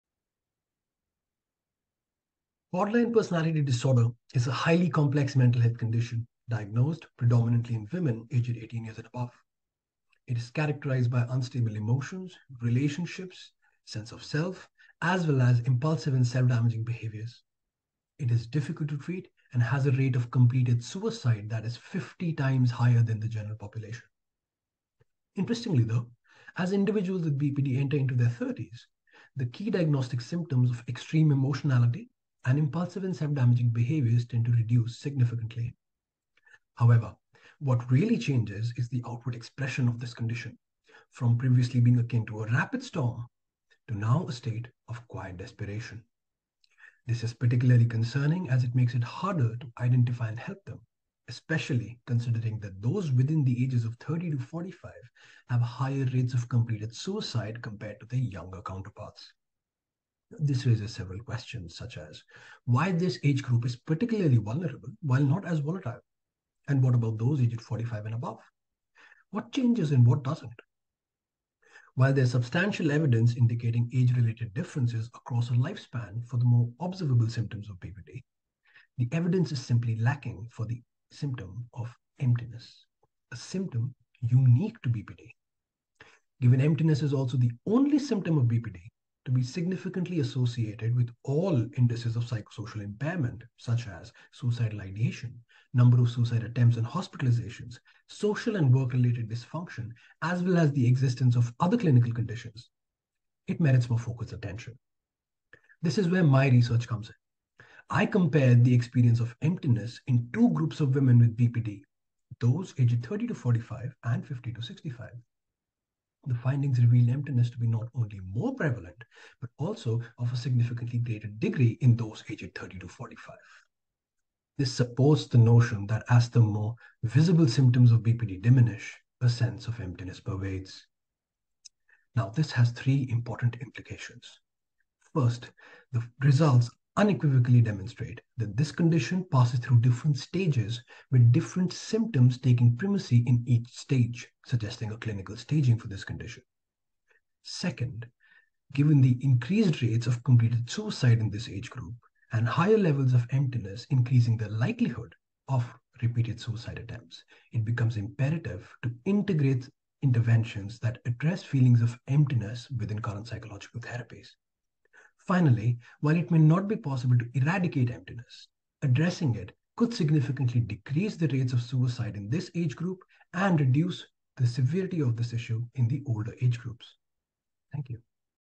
Experience of Emptiness in Women with Borderline Personality Disorder: An Age Comparison | 3MT 2023
Abhijatya Dhar, 3MT 2023 finalist for the School of Psychology